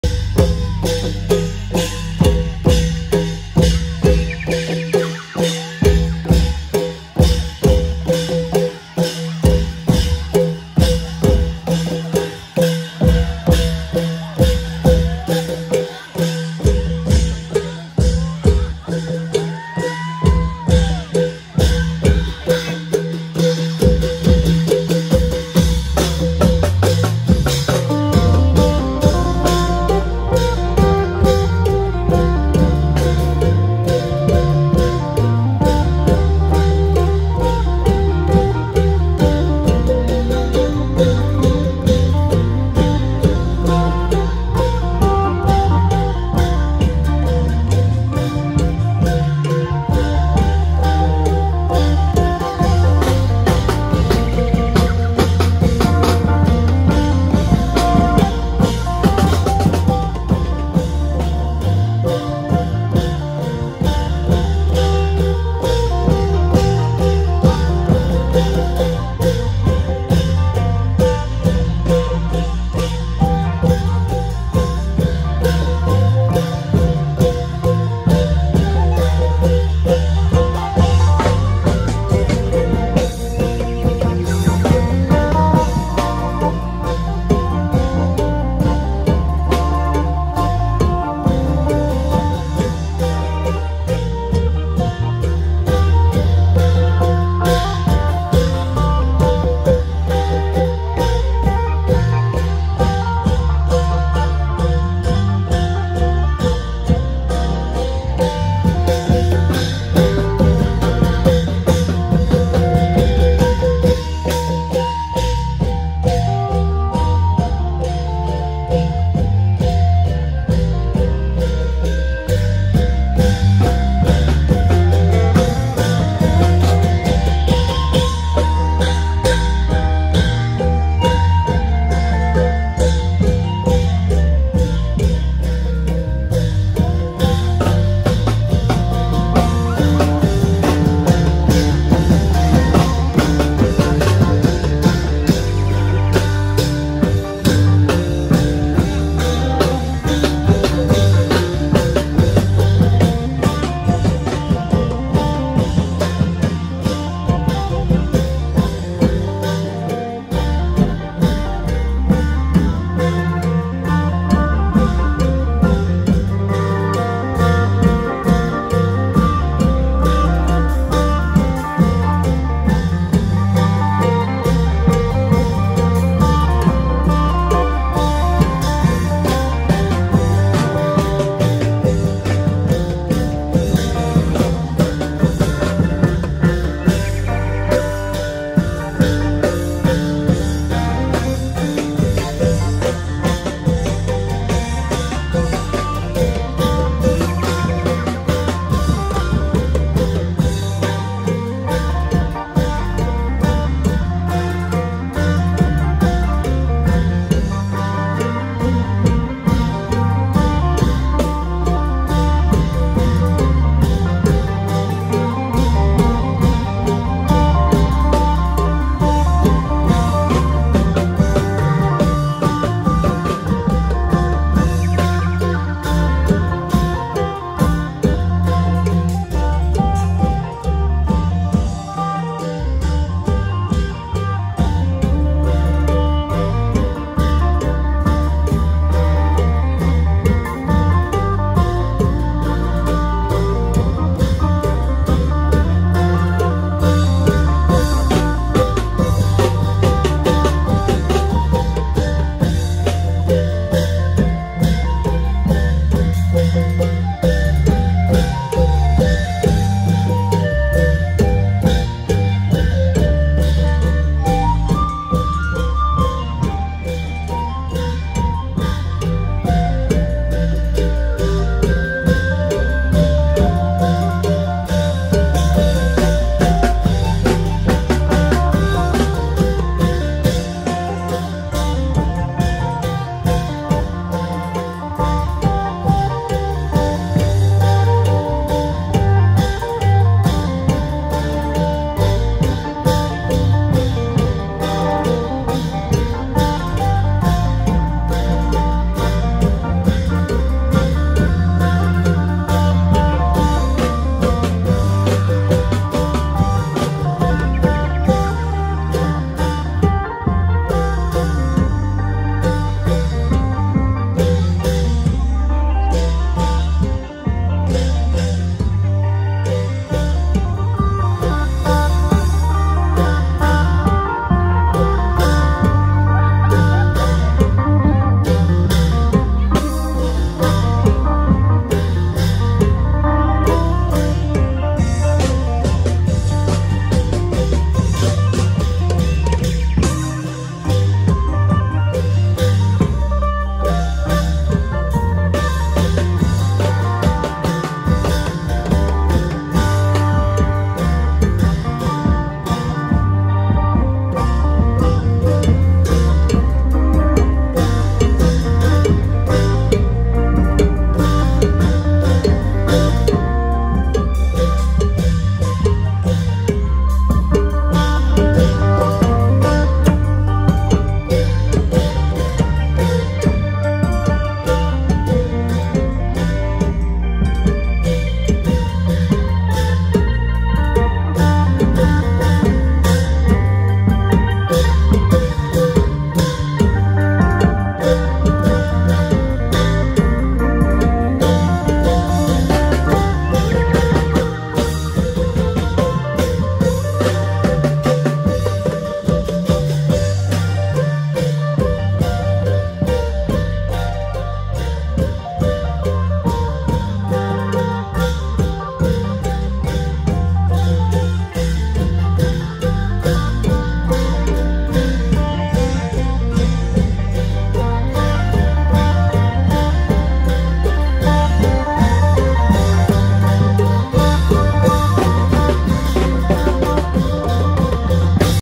จังหวะป่นกุ้ง ลายครวญฮักหวานๆ สังคีตอีสานกลองยาว บ.ดอกไม้ อ.สุวรรณภูมิ จ.ร้อยเอ็ด EP274
กดไลท์กดติดตามกดแชร์เป็นกำลังใจให้ด้วยนะครับกรุณาใส่หูฟังเพื่อเพิ่มอรรถรสในการรับชมรับฟังติดต่องาน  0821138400￼